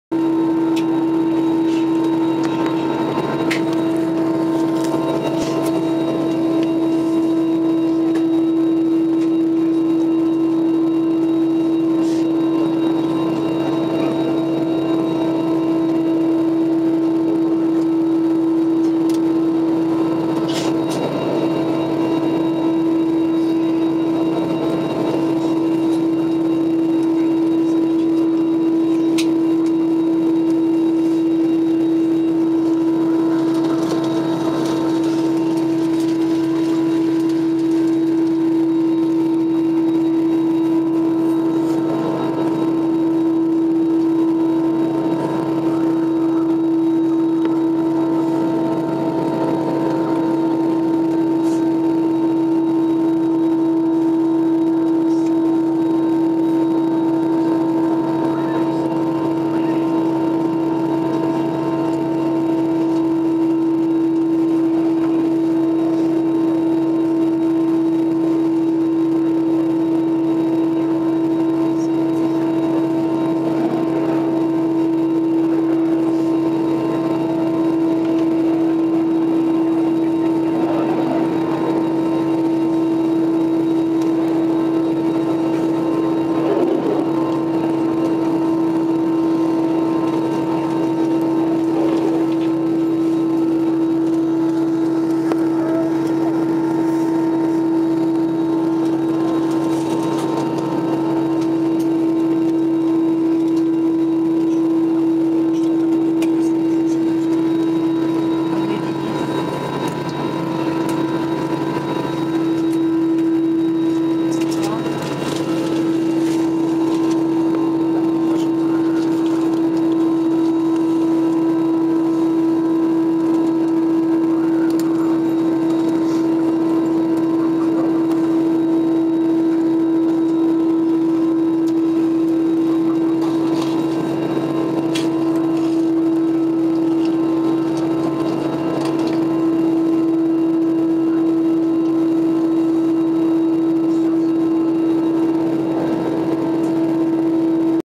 Deice Sheremetyevo A321 AFL SU36 090216
Противообледенительная обработка воздушного судна в аэропорту Шереметьево при выполнении рейса "Аэрофлота" SU36 на А321 9 февраля 2016 года